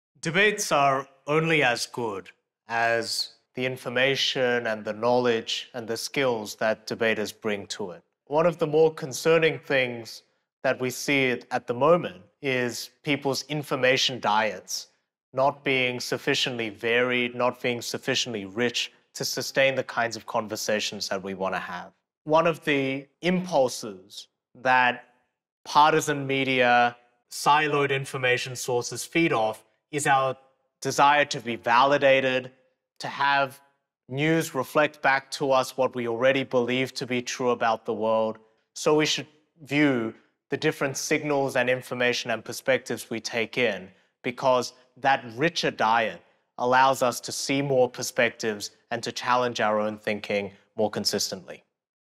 How to have better debates, according to Bo Seo
Train for any argument with Harvard’s former debate coach, Bo Seo.